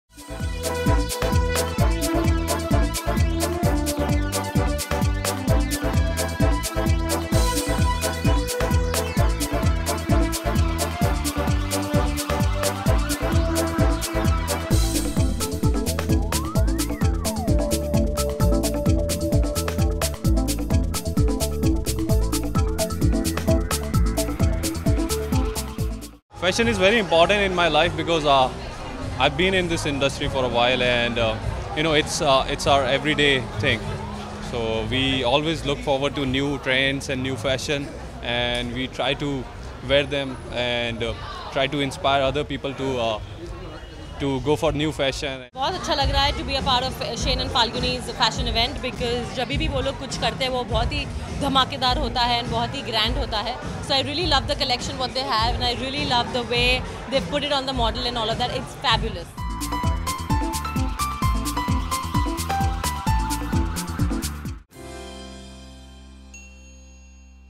Huma Qureshi and Elli Avram at Falguni Shane Summer Collection Launch
Bollywood actress Huma Qureshi looked stunning in a one-shoulder Falguni and Shane creation at the designers summer collection launch in Mumbai on January 31. Also many other bollywood celebs spotted.